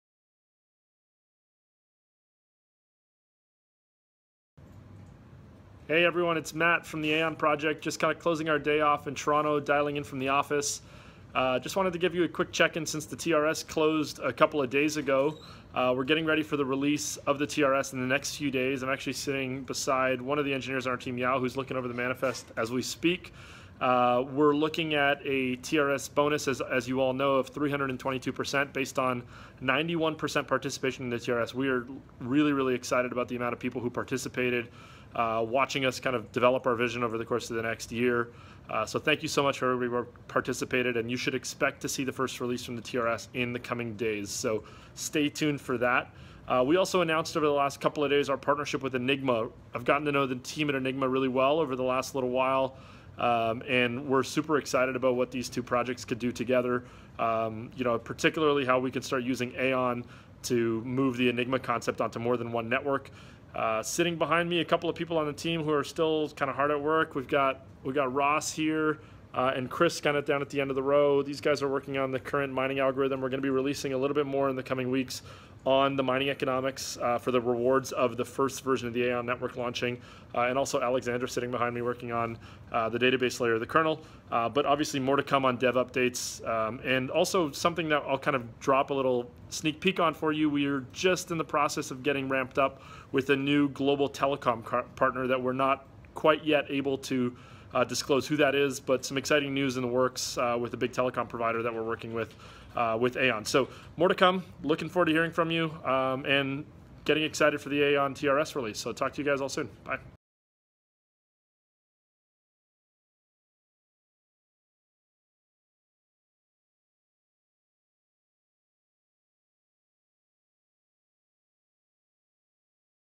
Matt Spoke - Weekly update for the Week of December 4
Aion Founder Matt Spoke discusses project updates for the Week of December 4